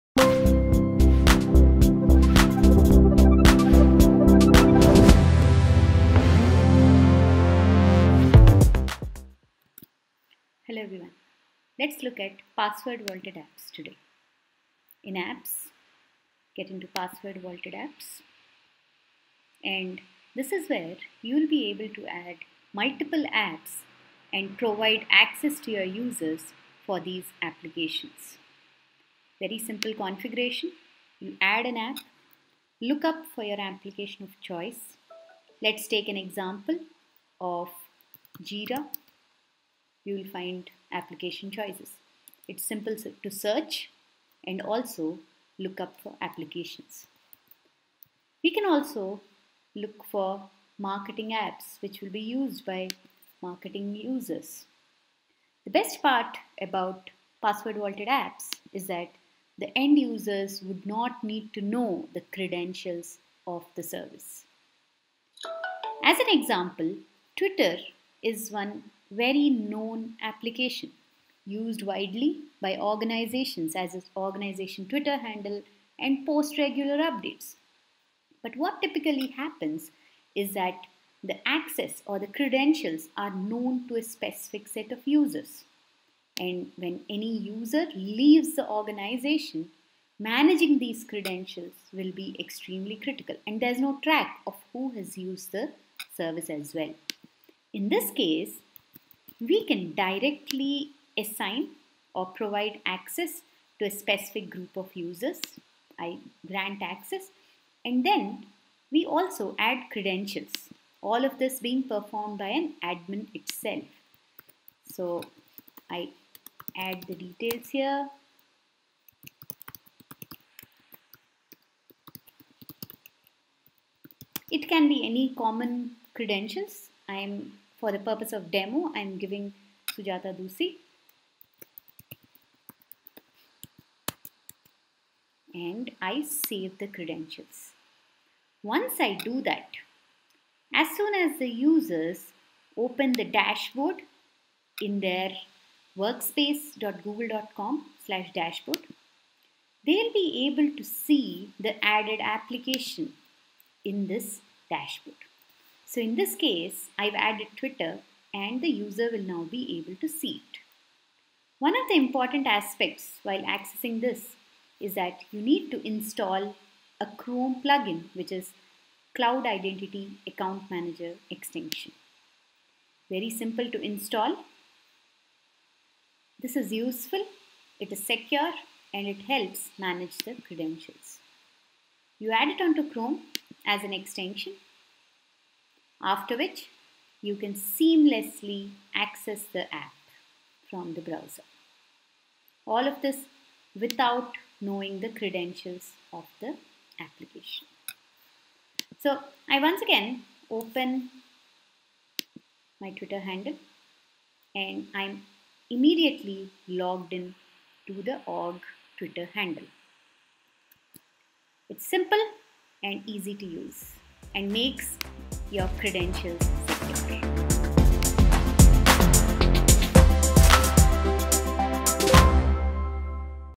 Google Workspace - Password Vaulted Apps
As an administrator, you can use the password vaulted apps service to manage access to some of the apps that don't support federation and that are available to users on the user Dashboard. The password vaulted apps service saves login credential sets for applications and assigns those credential sets to users through group association. When a user has access to one of these applications through a group, they can sign in to the application through the user dashboard, or they can sign in directly from the specific application. This functionality is possible by leveraging Chrome or Firefox extensions/plugins.